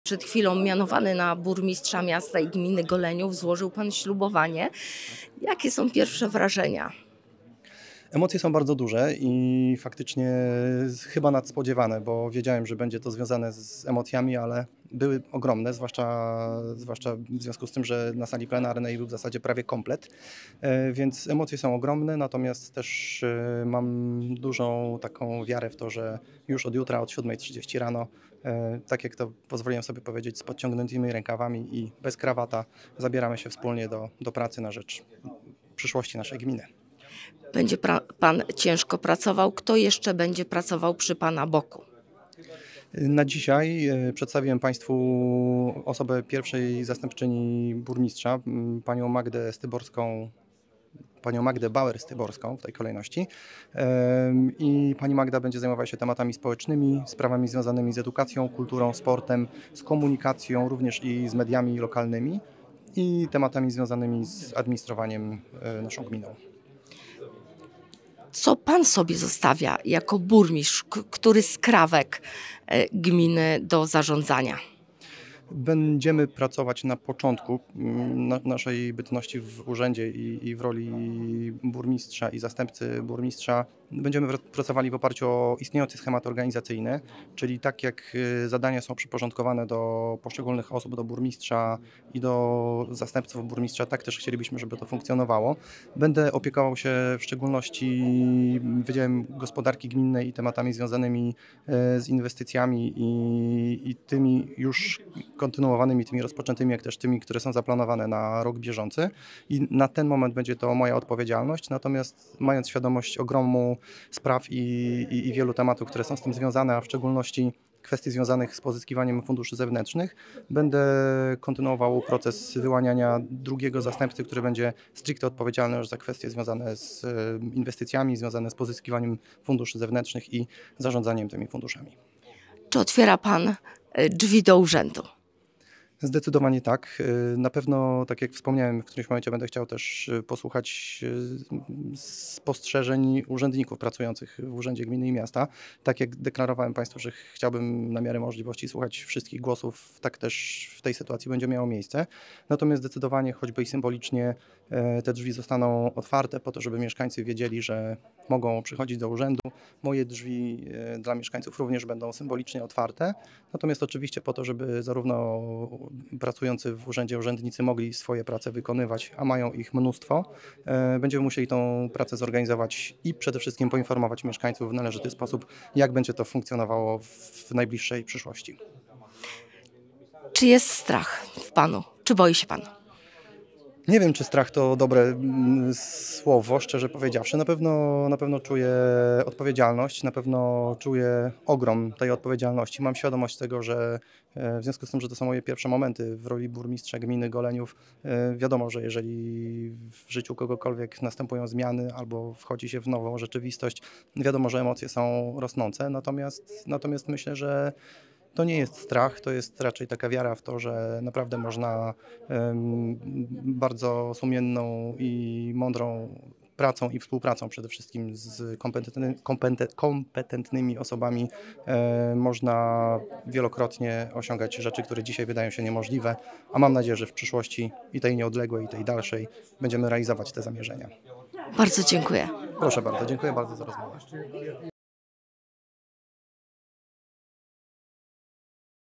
Pierwszy wywiad Burmistrz Krzysztof Sypień 7 maja 2024r.
🎙 ... Szybka rozmowa z Krzysztof Sypień - Burmistrz Gminy Goleniów tuż po zaprzysiężenia  ... 🎙